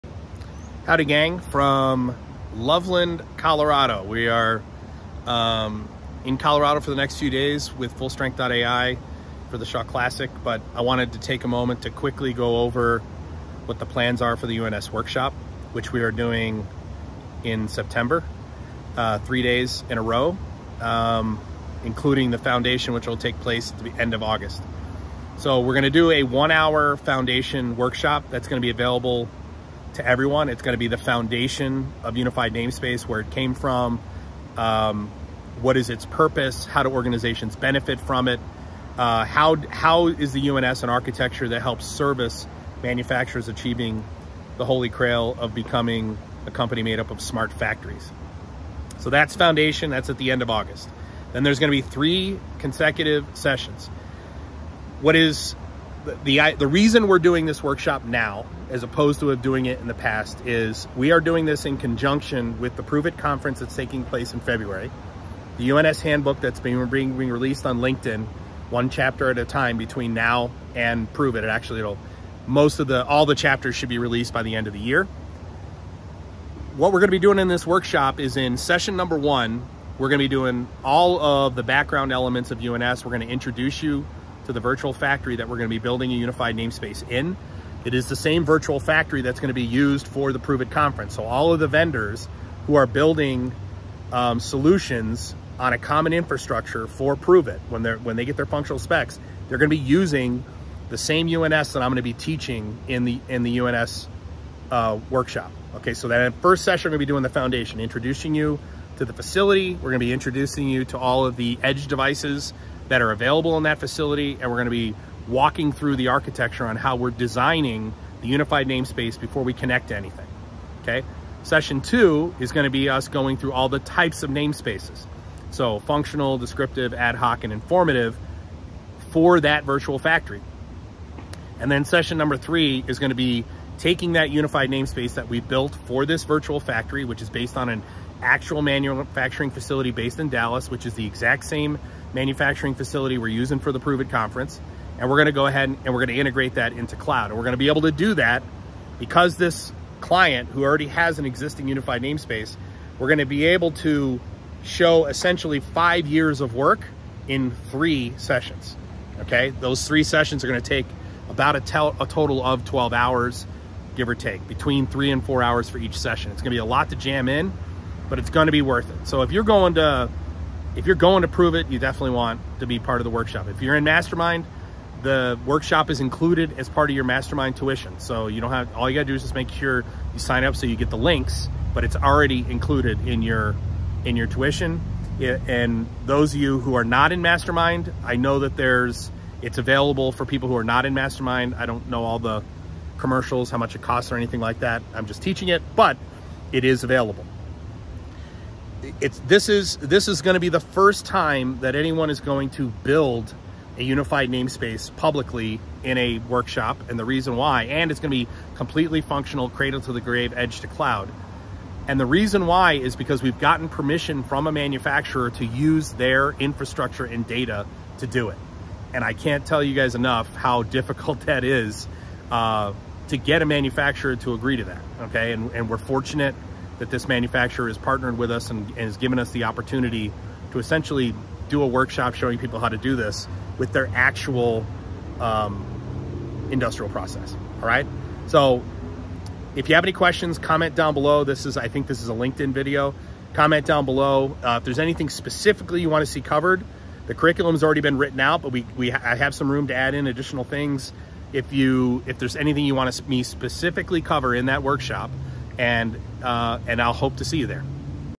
UNS Workshop — What to Expect
What to Expect from the 3-Day Unified Namespace (UNS) Workshop

Curious about our 3-Day UNS Workshop? In this video, we’ll walk you through what to expect, from hands-on sessions to expert-led instruction, and how it will equip you to master Unified Namespace in your facility.

Get ready to transform your operations and take your digital transformation to the next level!